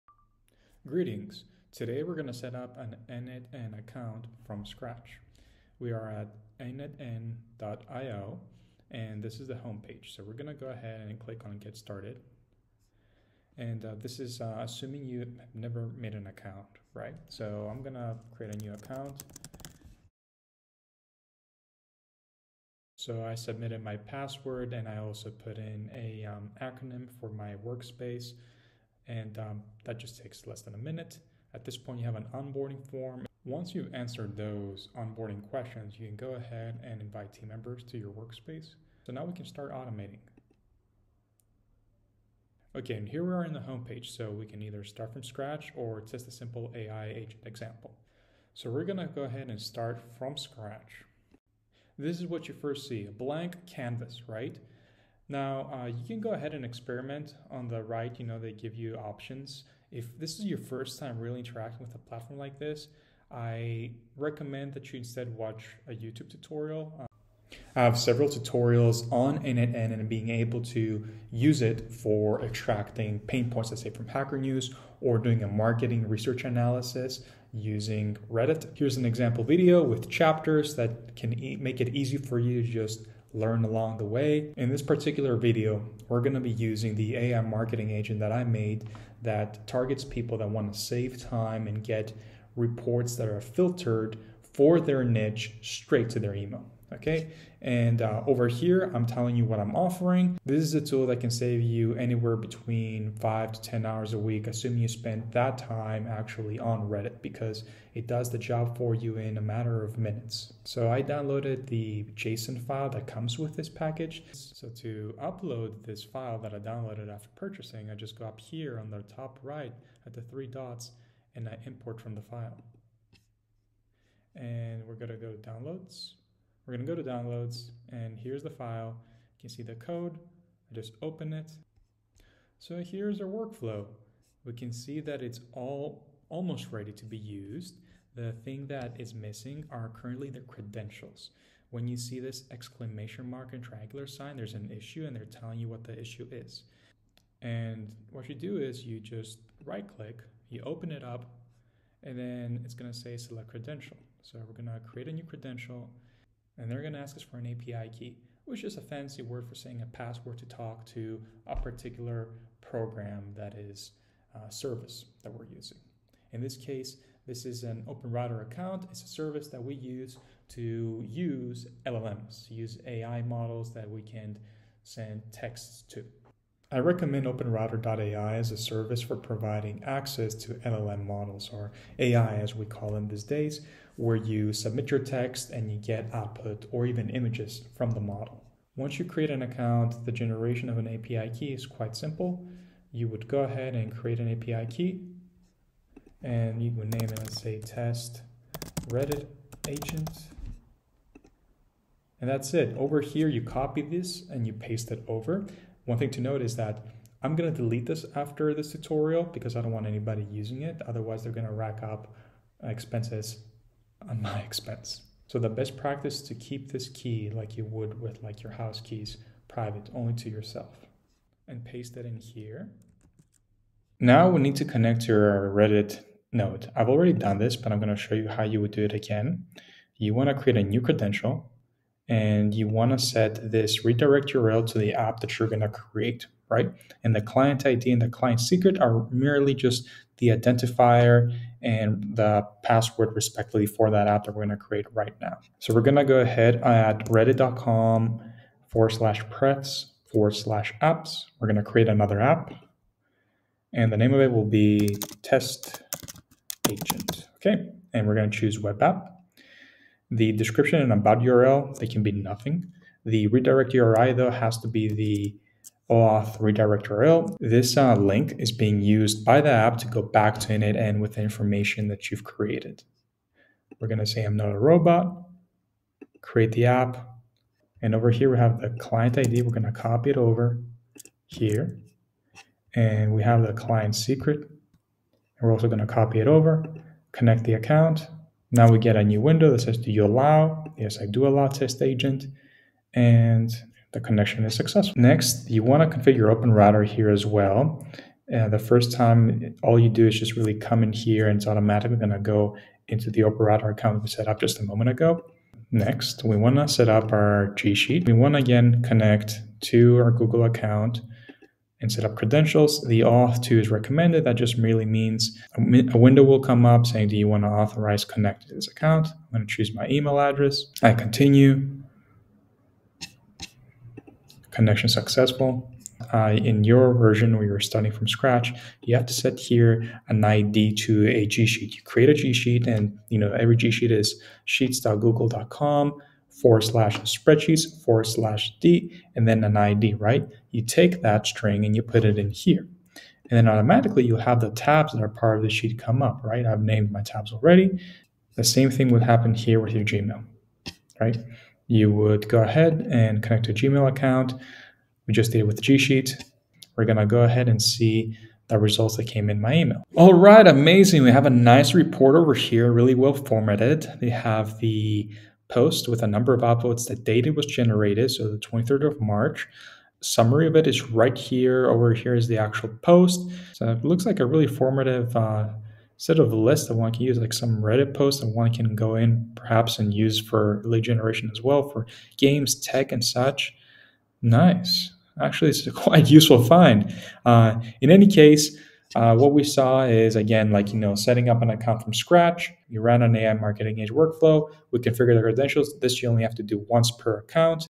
n8n account setup for beginners
This videos covers the setup of a n8n account from scratch. I also show you how to setup the following workflow